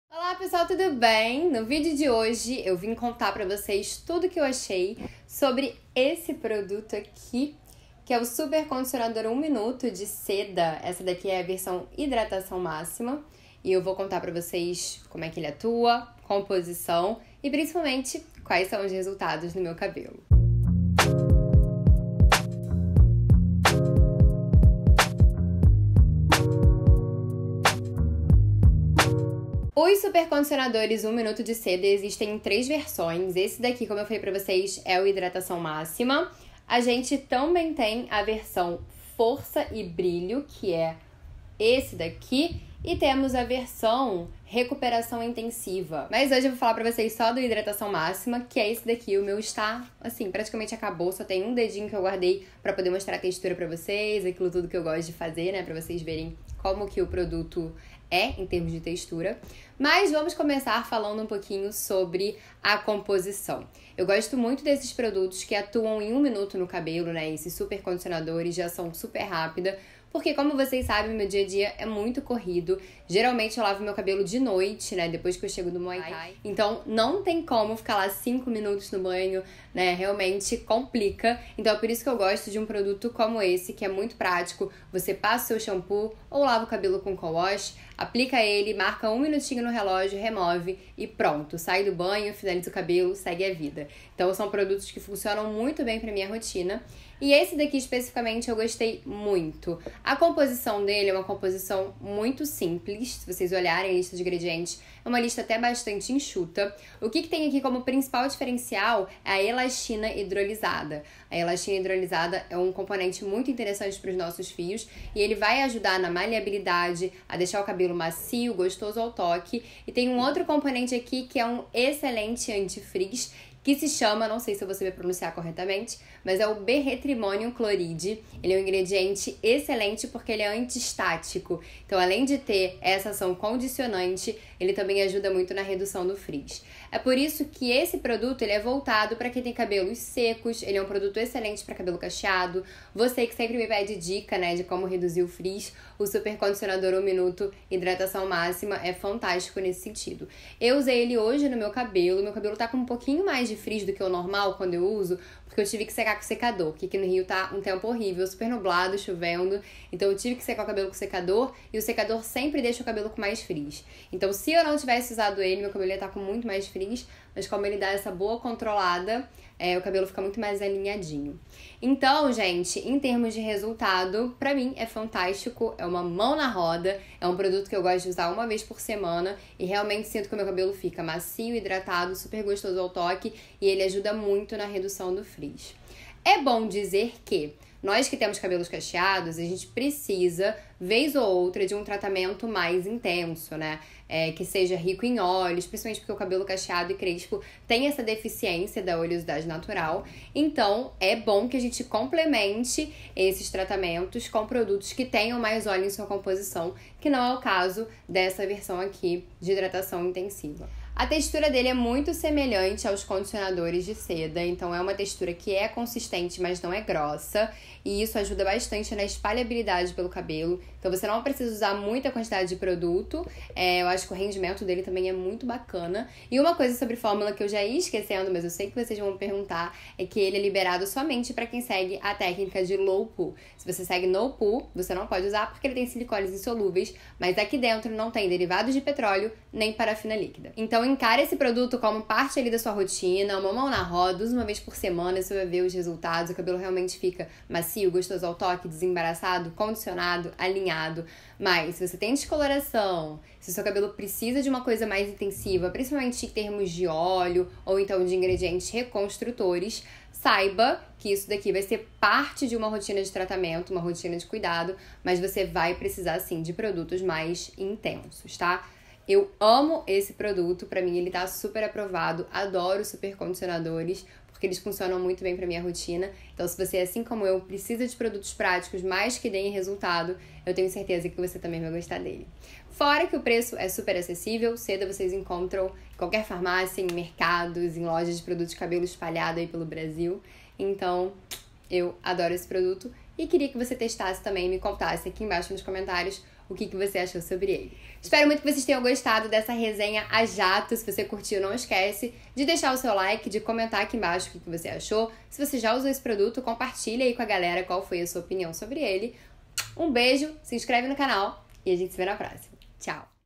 TUDO SOBRE O SUPERCONDICIONADOR DE SEDA - Hidratação Máxima*
Hey pessoal! No vídeo de hoje eu conto para vocês tudo que achei sobre o Supercondiconador de Seda - Hidratação Máxima. Ele promete cabelos mais hidratados e macios em apenas 1 minuto. Será que dá certo? A composição é boa? Te conto tudo isso no vídeo ;) Espero muito que gostem! ♥
* Assista em HD e expanda para mais informações! *